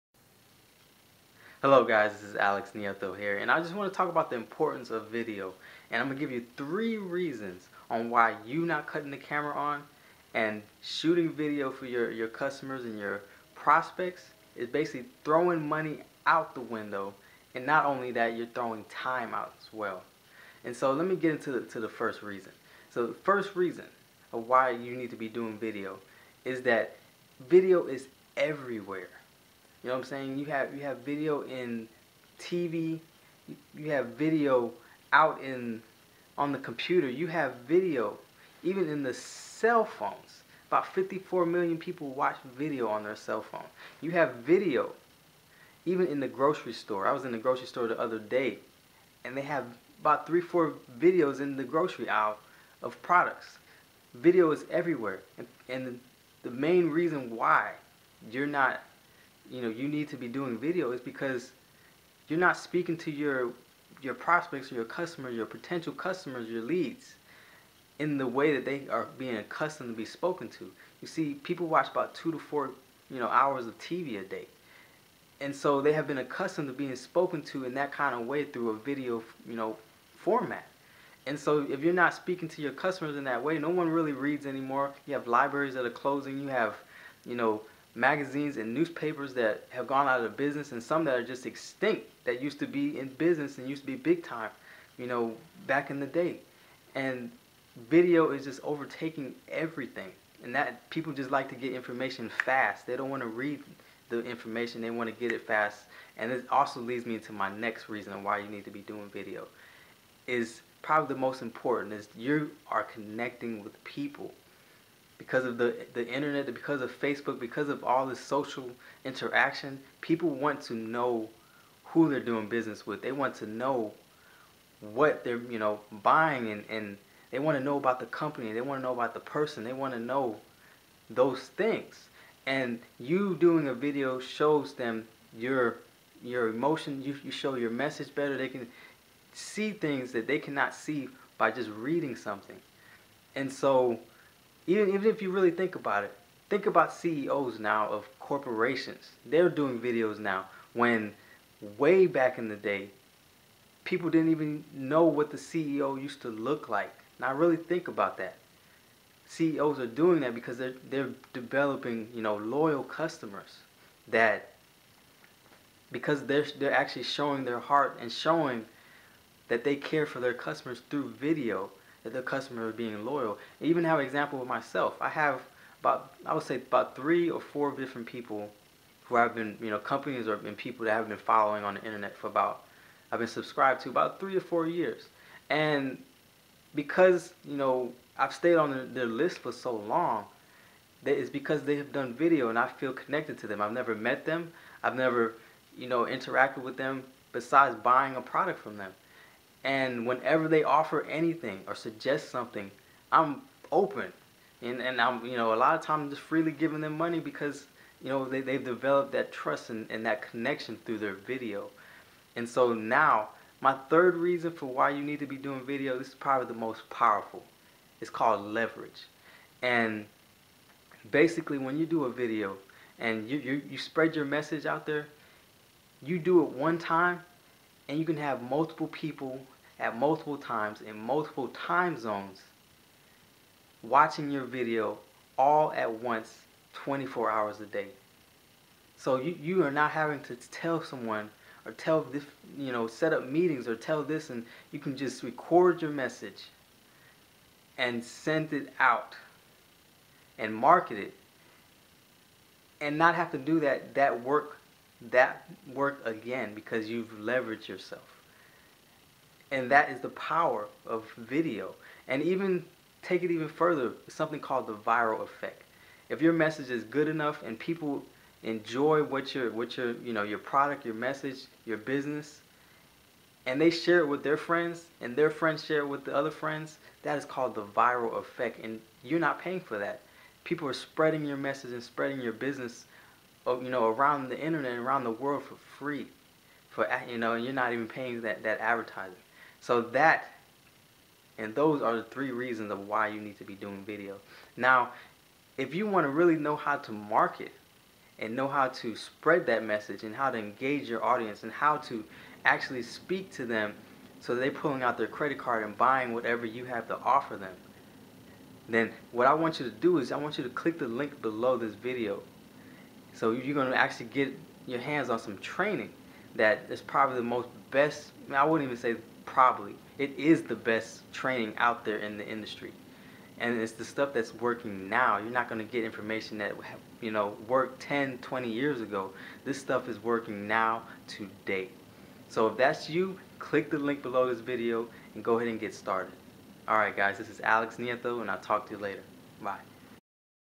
3 Reasons How Building Your Business Without Video Marketing Is COSTING You Time And Money
If you aren't using video to market your business you are leaving a lot of money on the table.  Making can be intimidating but not doing it can cost you more. In this video Alex Nieto explains 3 reasons why that is.